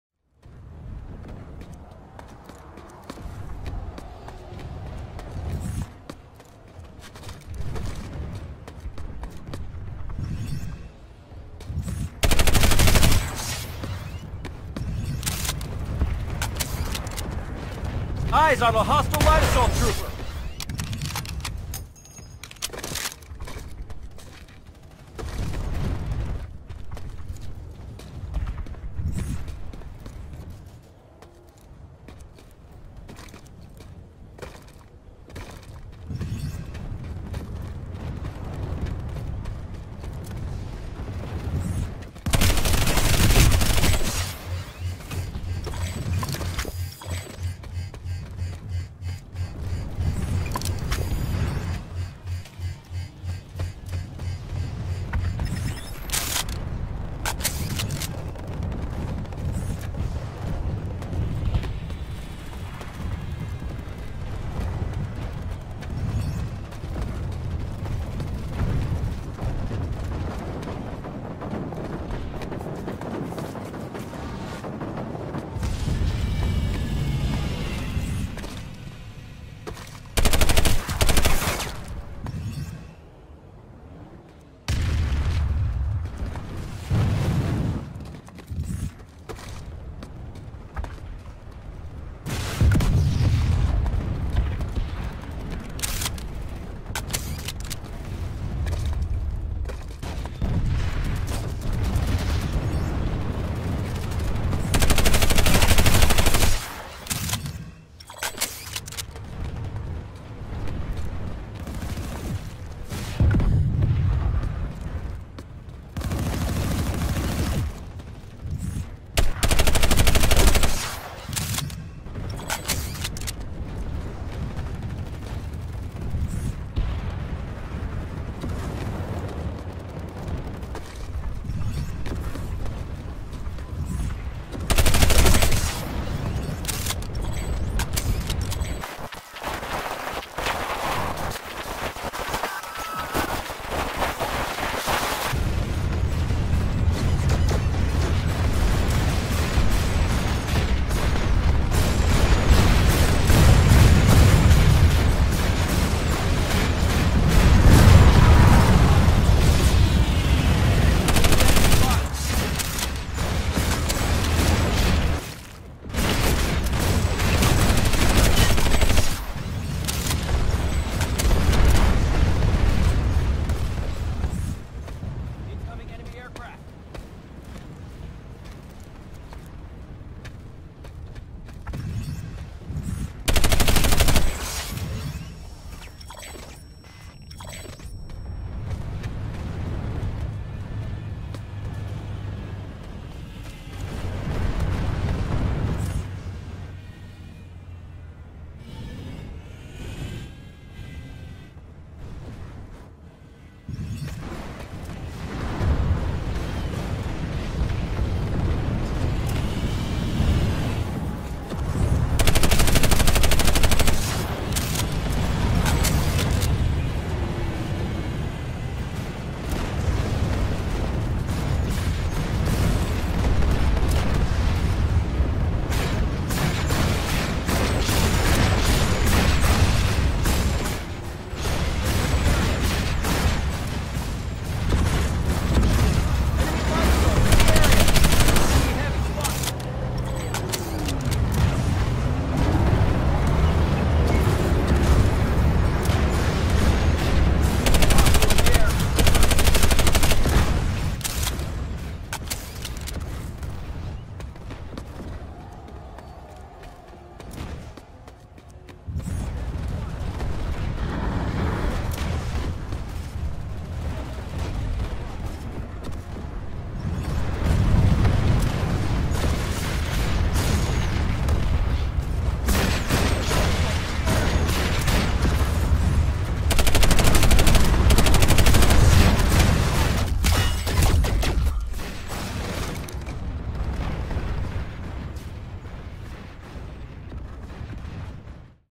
More SMG Infiltrator fun
Big battle over Crossroads Watchtower